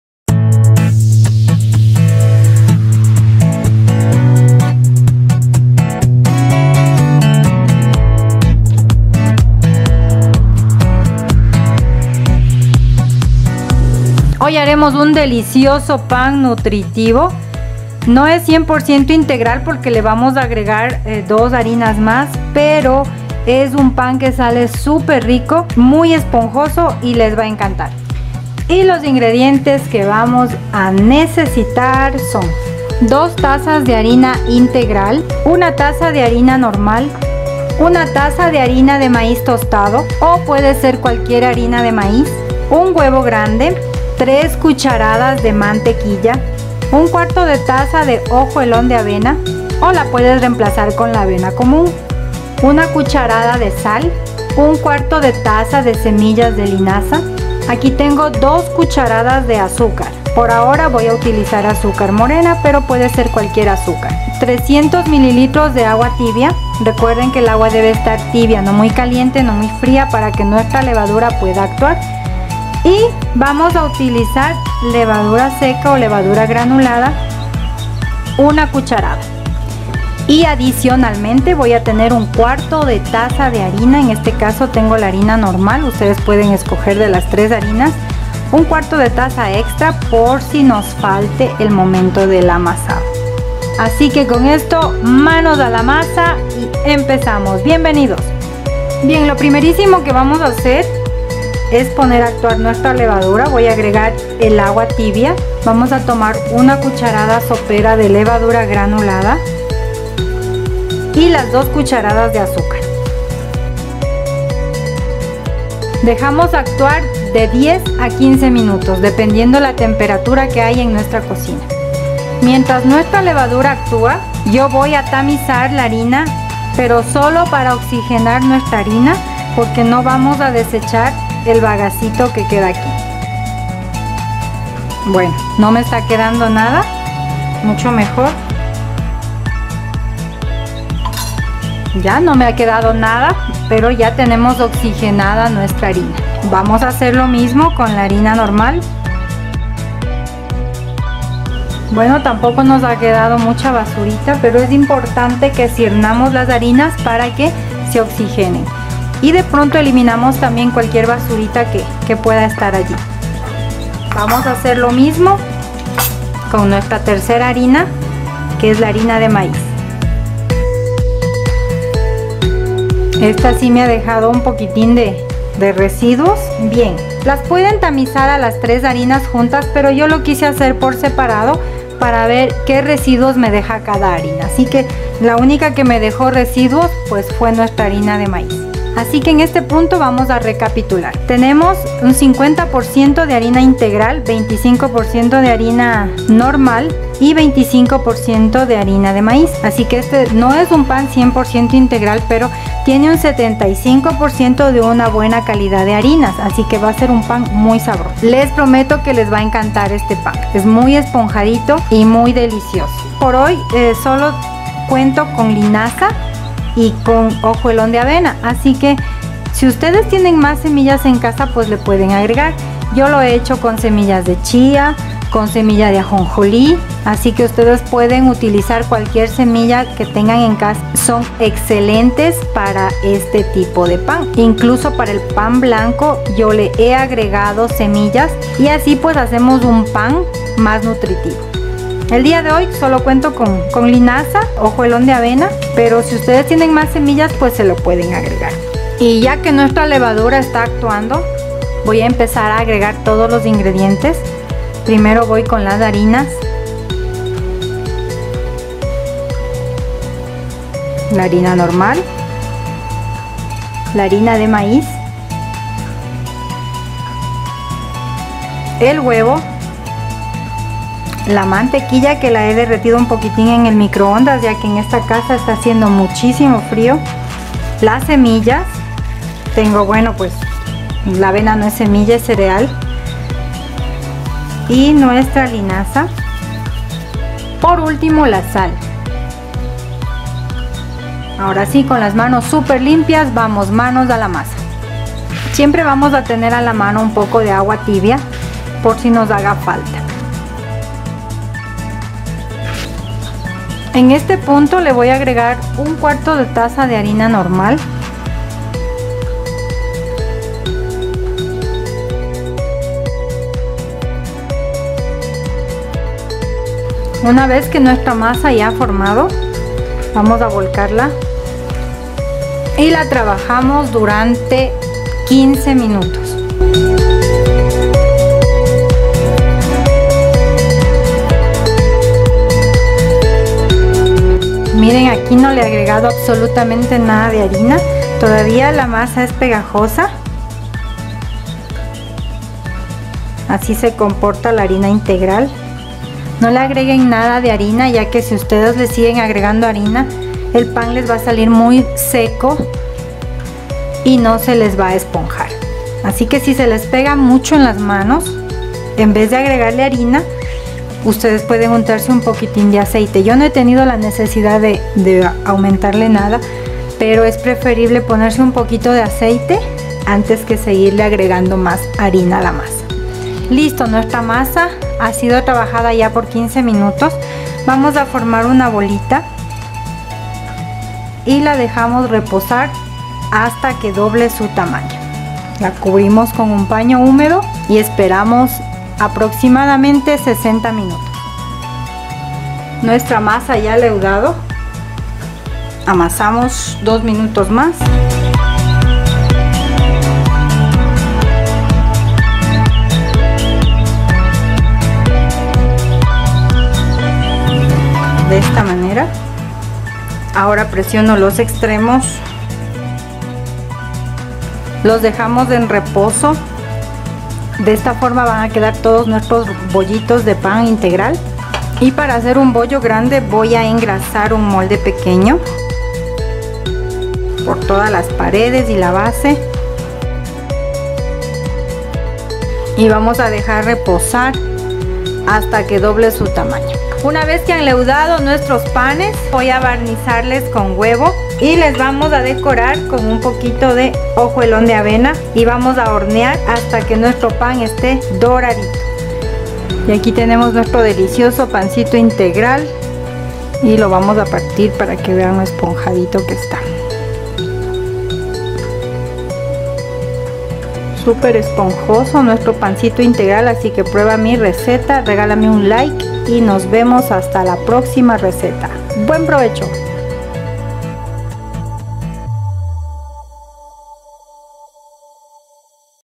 Pan Integral...Receta original..
Este pan está súper delicioso y esponjoso...con Harina integral , harina de maíz, hojuelón de avena y semillas de  linaza para hacerlo más nutritivo...Atrévete a hacerlo!!!

Beneficios del pan integral:
El pan integral es más nutritivo. ...
Pan integral, un remedio natural contra el estreñimiento. ...
El pan integral favorece la digestión. ...
El pan integral ayuda a controlar el peso. ...
El pan integral reducen el riesgo de cáncer. ...
Ayuda a fortalecer nuestros sistema inmune. ...
Protege tu corazón. ...
El pan integral ayuda a regular el azúcar.